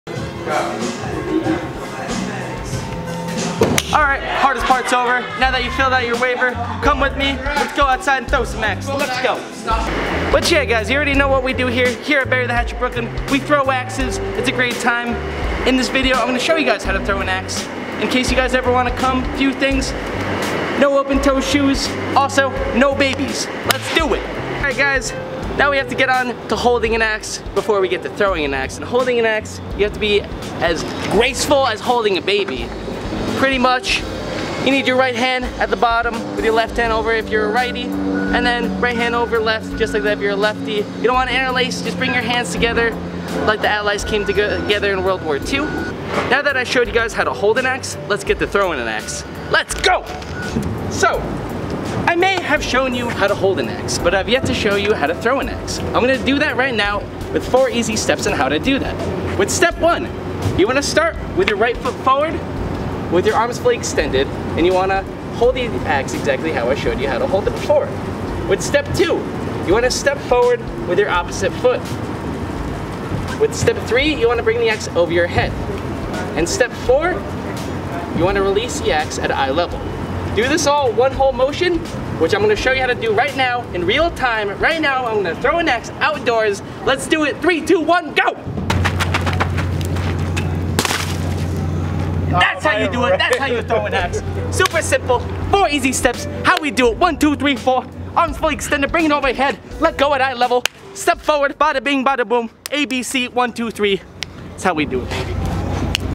How To Throw an Axe
This video goes over some simple techniques on how to throw an axe. How To Throw An Axe was filmed on location at our Brooklyn Location using our internal employees for production, filming, editing, and acting. We look forward to bringing more content like this in the near future.

(No watermelons were harmed in the filming of this video)

Video Credits

Edward Lopata
Joseph Galvano
Matthew Sylvester
Daniel Gonzales
Micheal Barnette
Video Description
How To Throw An Axe is a video about Axe Throwing. What is Axe Throwing?
Axe throwing is a sport in which the competitor throws an axe at a target, attempting to hit the bullseye as near as possible. Axe throwing is an event in most lumberjack competitions. Competitive axe throwing is becoming increasingly popular in the United States. Quite simply, it’s like playing darts but with axes.